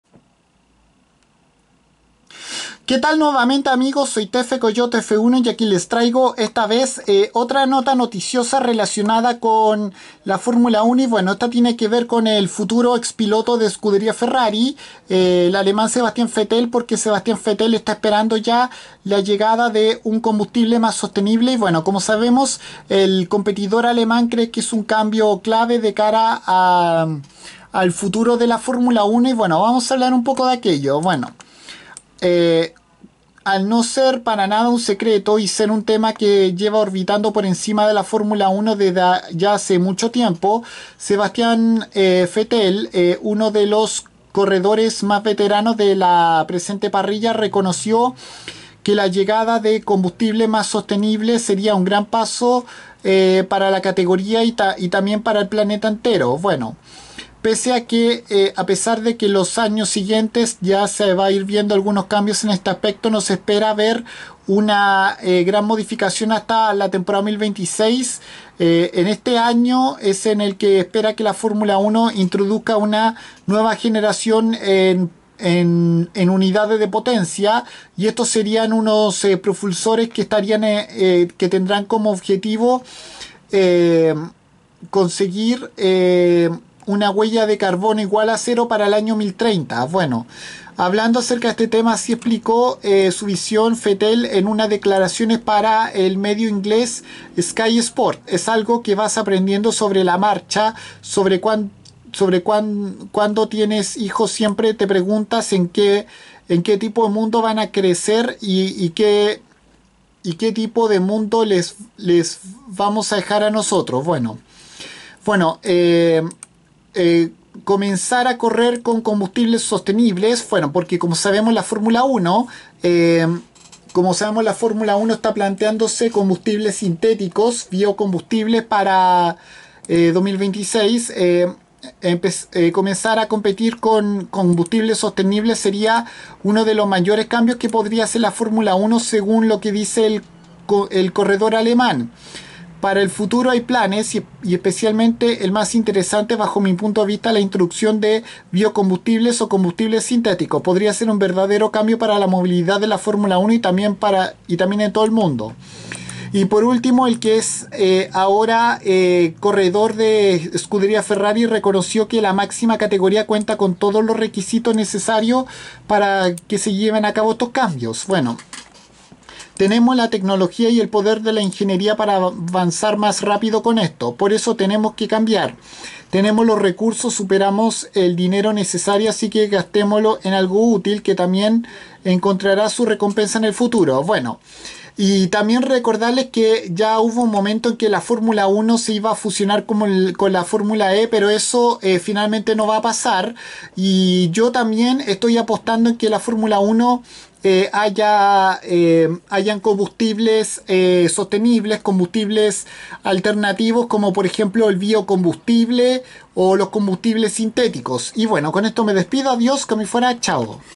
Sebastian Vettel espera la llegada de los combustibles sostenibles a la Fórmula 1
El competidor alemán futuro ex piloto de Scuderia Ferrari cree que si el Gran Circo implemente combustibles sustentables sería un gran paso de cara a su futuro